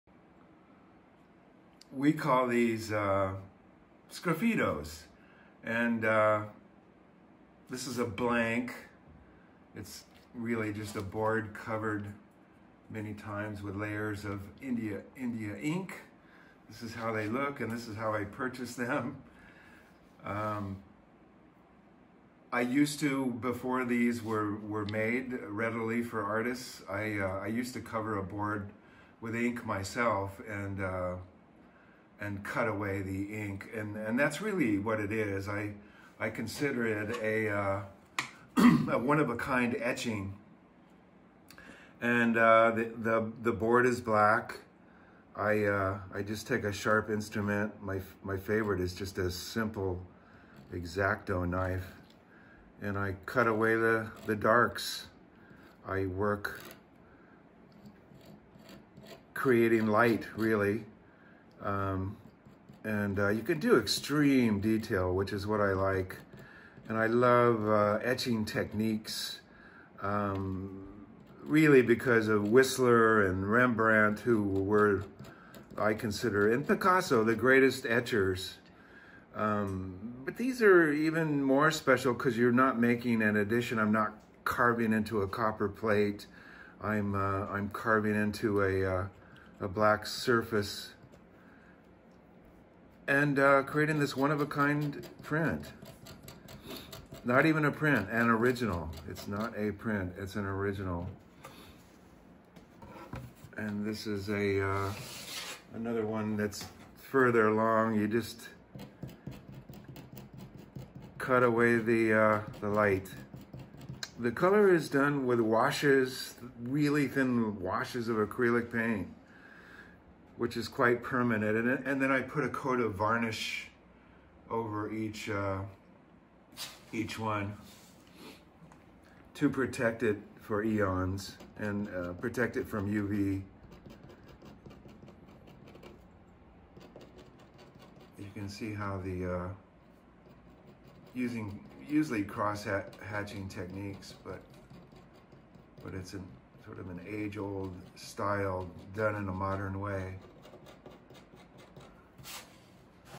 Creating Scraffito Etching Originals By Robert Lyn Nelson
Robert Lyn Nelson explains how he creates his scraffito etching one of a kind originals. This is a unique media that he's used to create art of many different subject matter from portraits and figures to still life in different styles including cubism.